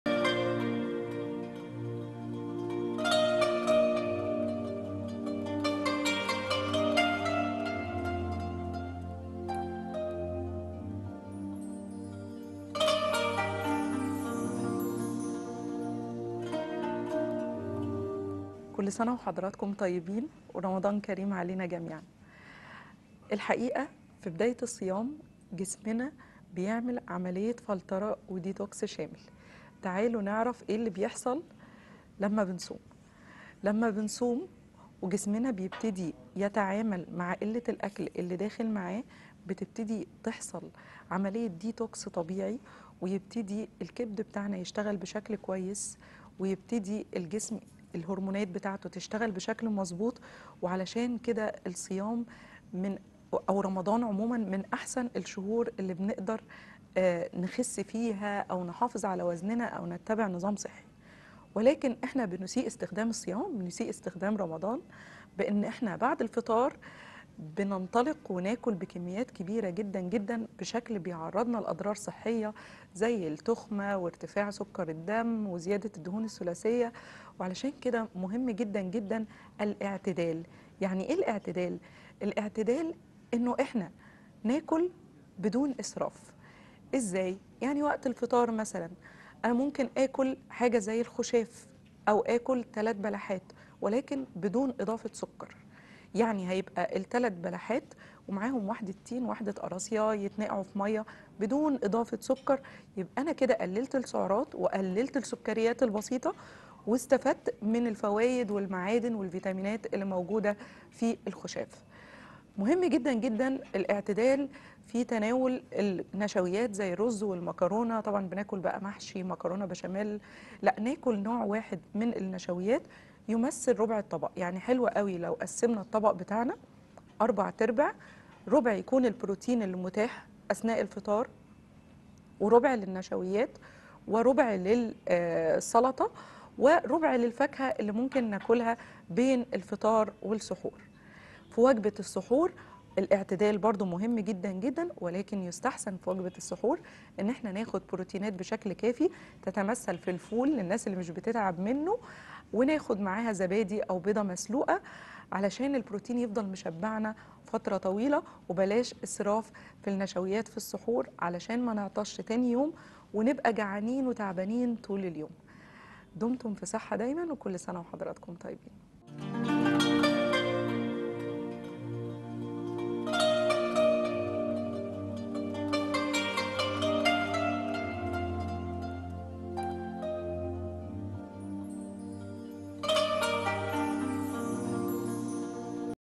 صحتك فى رمضان 29-3-2023 - نصائح طبية للتغذية الصحيحة في رمضان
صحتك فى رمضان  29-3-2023
نصائح طبية للتغذية الصحيحة في رمضان
 د / سمر حمدون  - استشارى التغذيـة